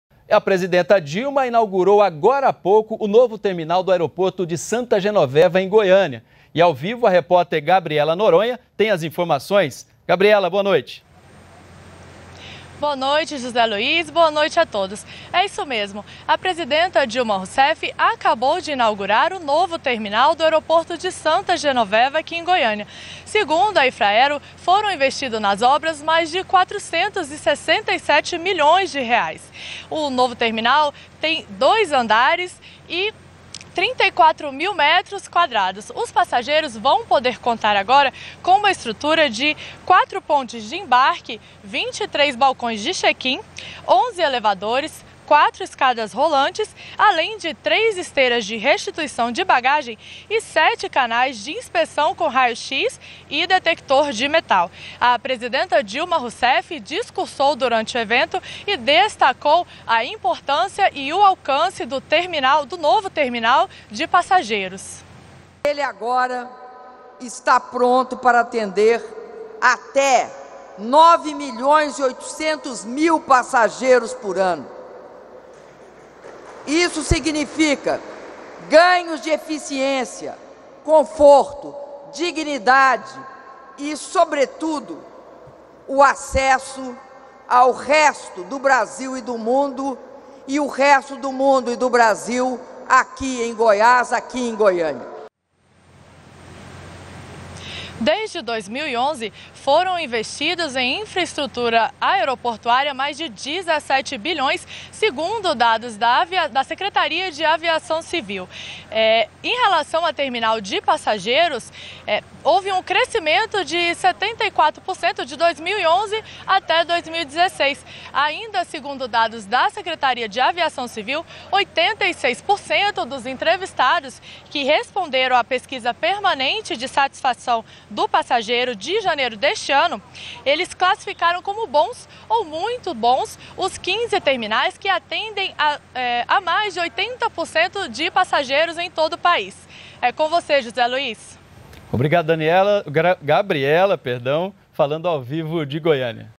Dilma inaugura novo terminal de aeroporto goiano
JORNAL NBR - 09.05.16: Nesta segunda-feira, a presidenta Dilma Rousseff inaugurou em Goiânia o novo terminal do aeroporto de Santa Genoveva. Mais de R$ 467 milhões foram investidos nas obras. Em discurso, a presidenta destacou a importância do novo terminal, que pode agora receber até 9,8 milhões de passageiros ao ano.